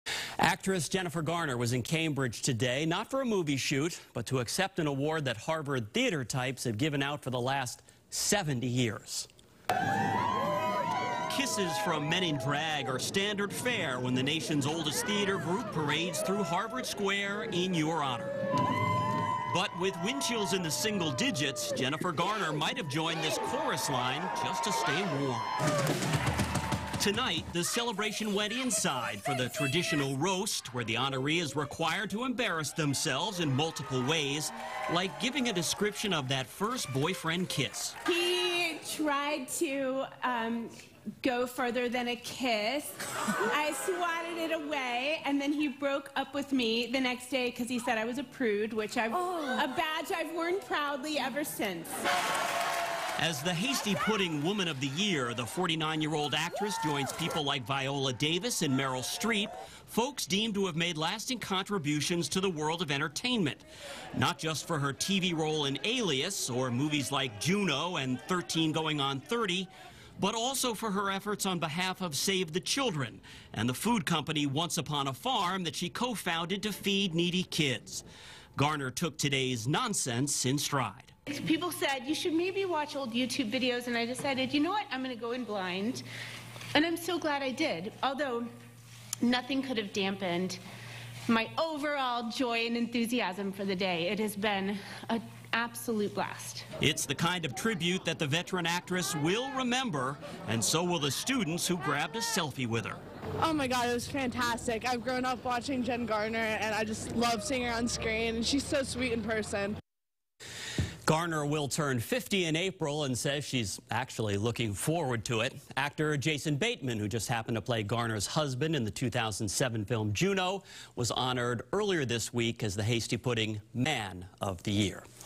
Jennifer Garner Celebrated As Hasty Pudding's Woman Of Year
“Alias” actor Jennifer Garner was honored as Hasty Pudding Theatrical’s Woman of the Year at a parade in historic Harvard Square Saturday afternoon.